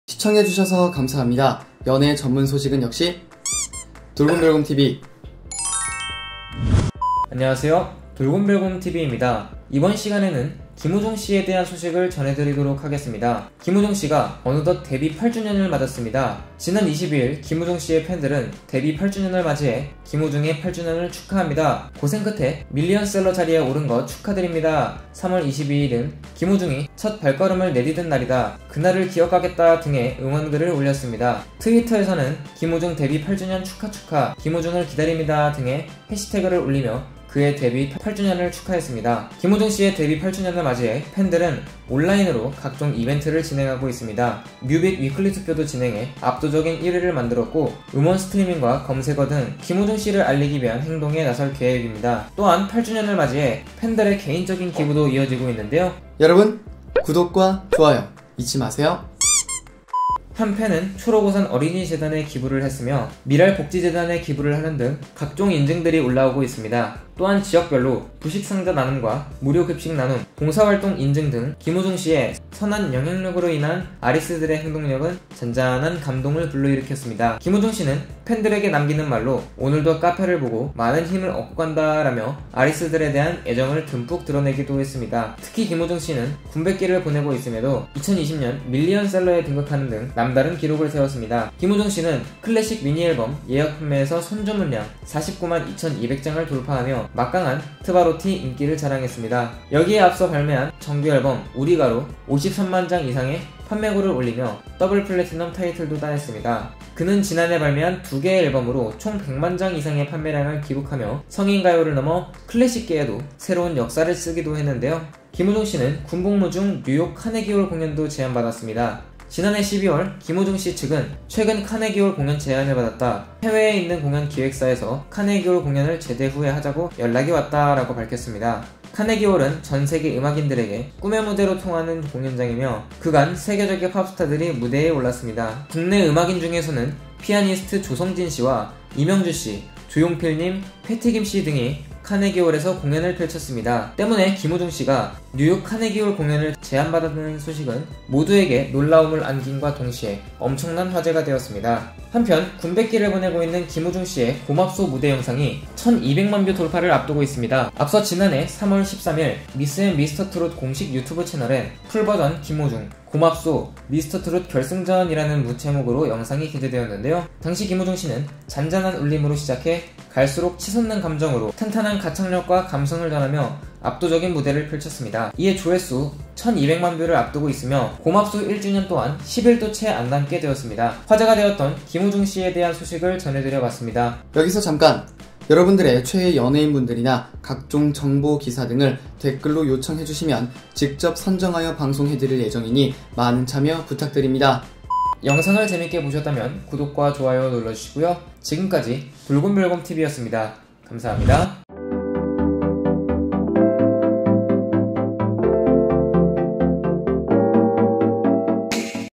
【ENG】트바로티 김호중, 데뷔 8주년 맞이해 다양한 이벤트 펼쳐졌다! Kim Ho-joong 돌곰별곰TV
정대규

감사합니다.

네이버 파파고 다국어 번역 :

Tbarotti Kim Ho-joong celebrated his 8th anniversary with various events!

キム·ホジュン、デビュー8周年を迎え様々なイベントが行われた！

T-BARO T卹金浩中，迎接出道8週年，展開了多樣的活動！